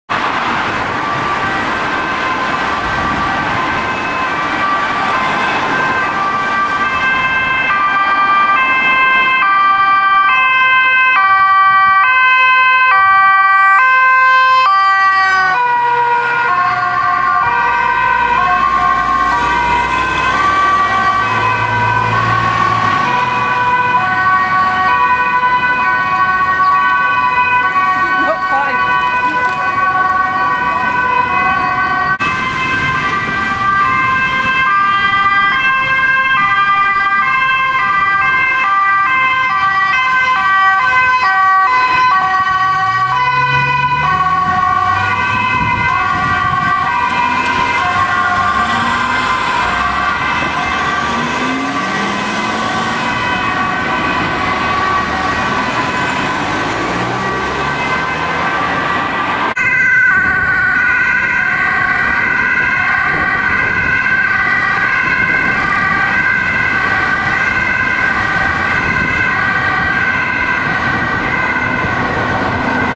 Politie & Ambulance met spoed in Amsterdam { Versneller }
Gefilmd op 6-2-2023 Amsterdam
1 politie Mercedes Vito en B klasse
en Ambulance Spacilty Care Ambulance 13-162